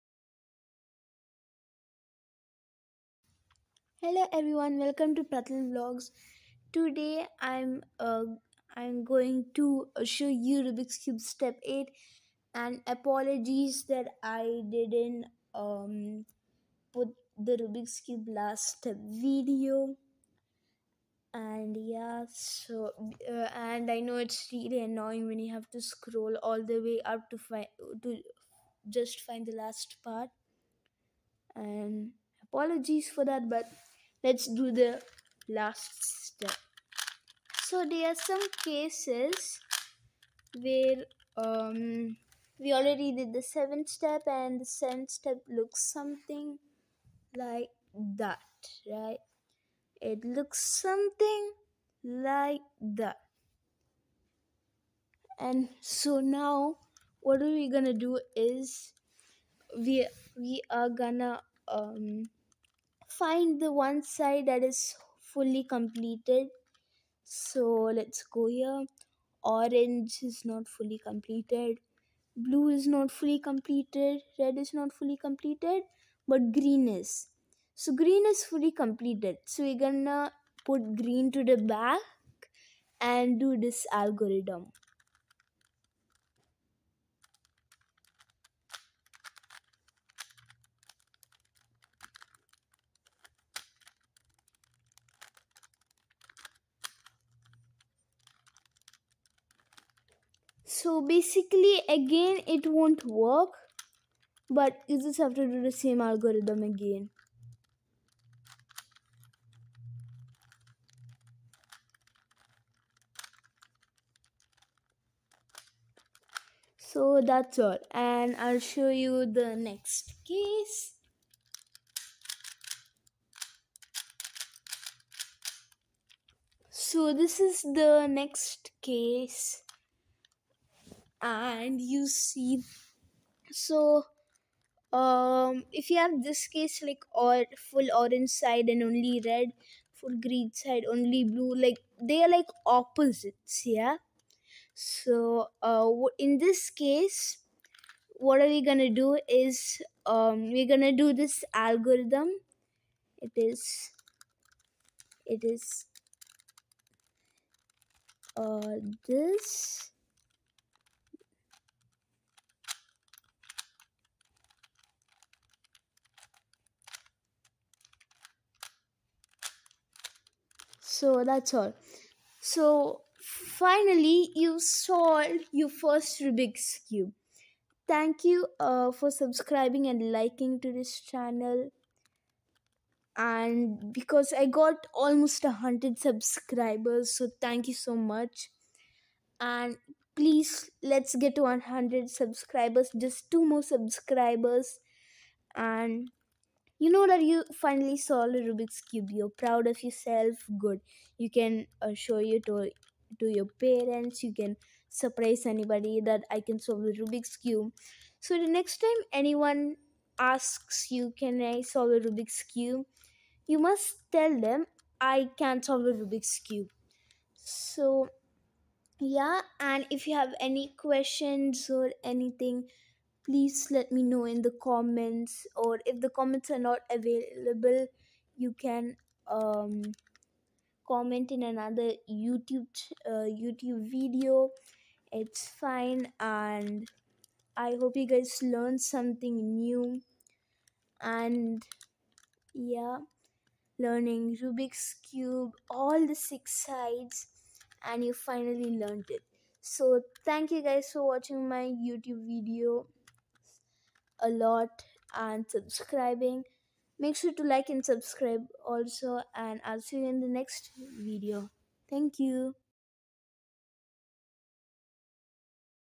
How to solve a 3×3 rubix cube step 8 (Final Step!)
Last step of solving a 3×3 Rubix Cube!